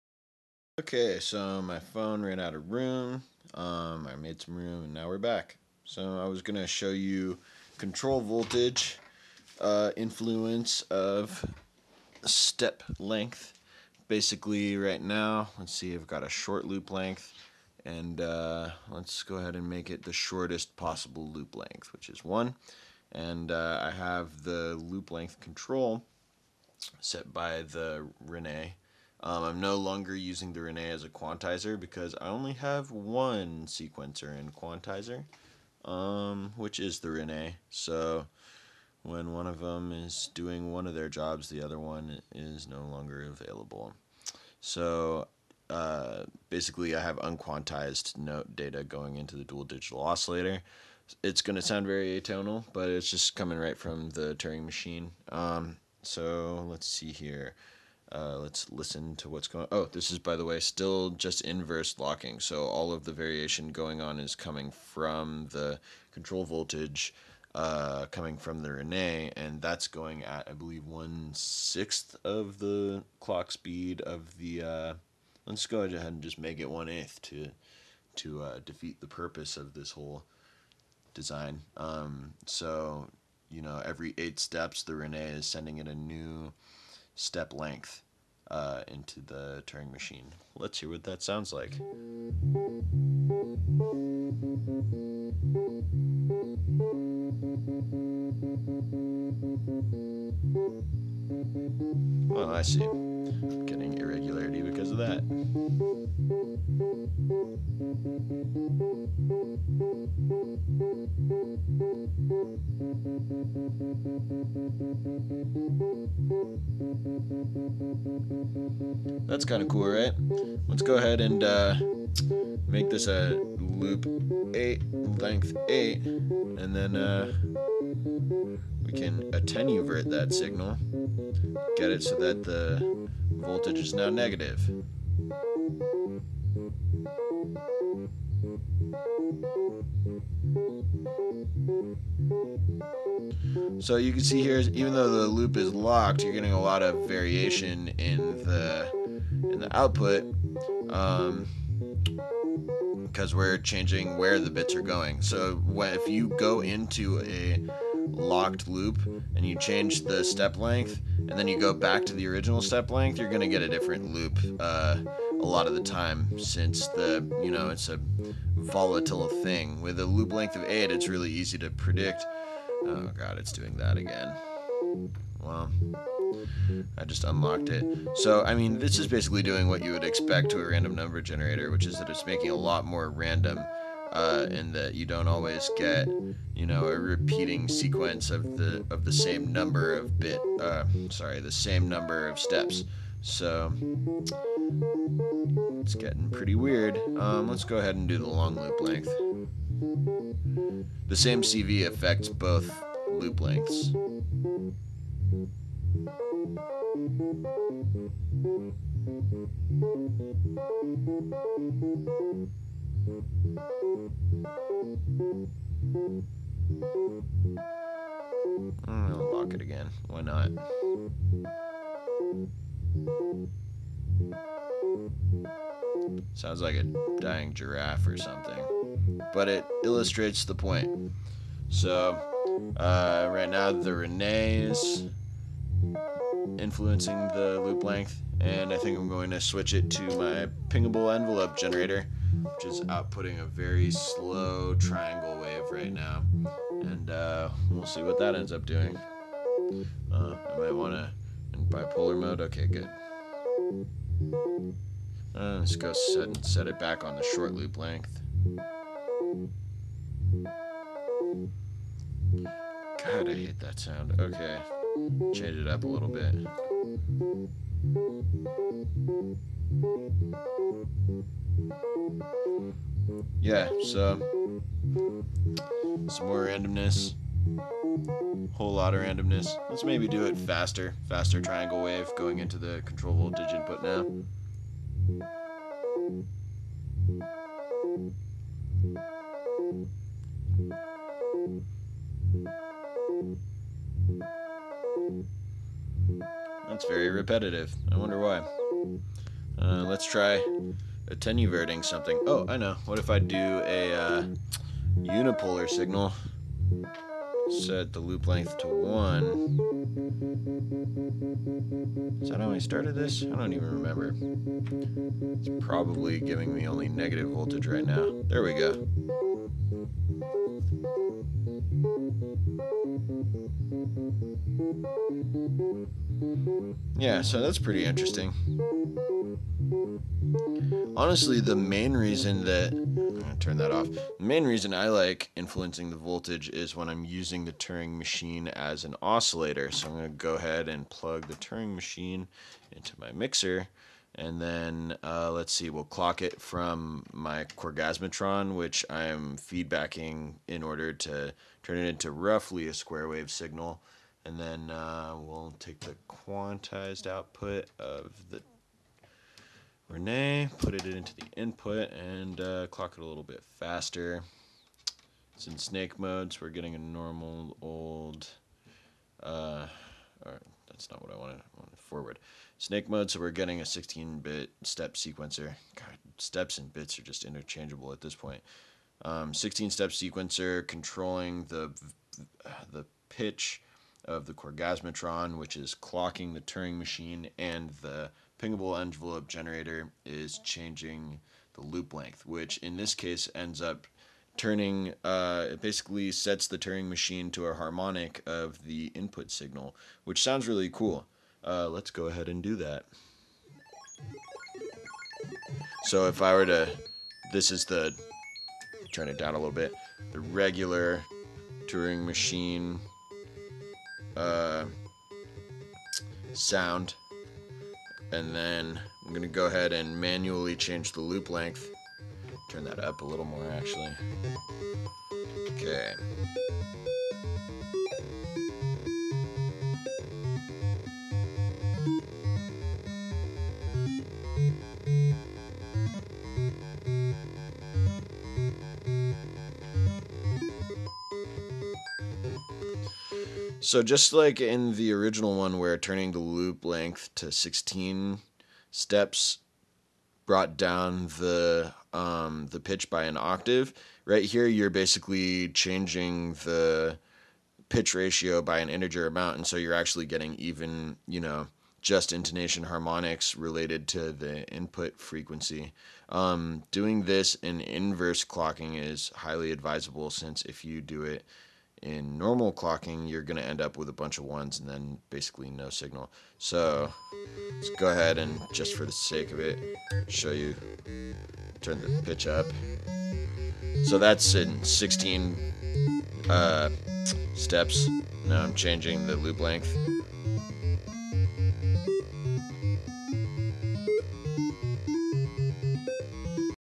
Turing Machine bytes expander under control voltage
showing the application of control voltage to the bytes turing machine expander - does pretty much what you would expect, changes the length of the sequence with a voltage.